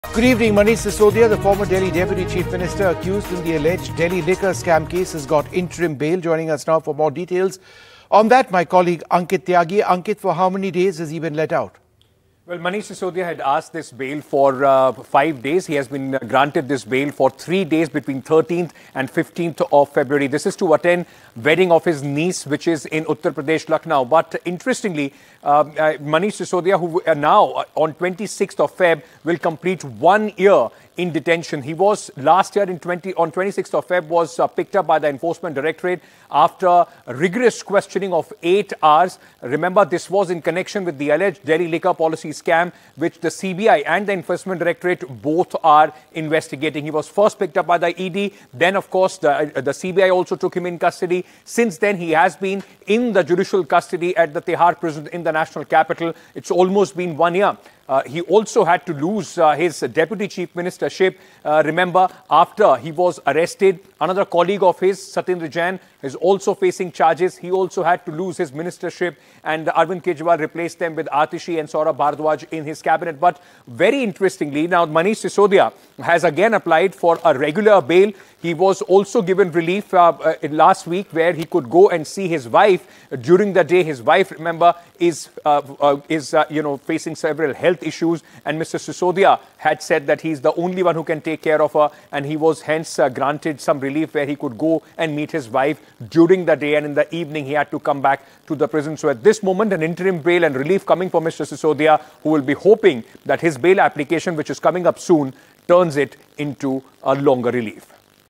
Manish Sisodia Gets 3-Day Bail To Attend Niece's Wedding
A Delhi court on Monday granted three-day interim bail to senior AAP leader Manish Sisodia, arrested in the alleged excise policy "scam", to attend his niece's wedding. Special judge MK Nagpal granted the relief to Manish Sisodia from February 13-15 in the corruption and money laundering cases being probed by the CBI and ED, respectively.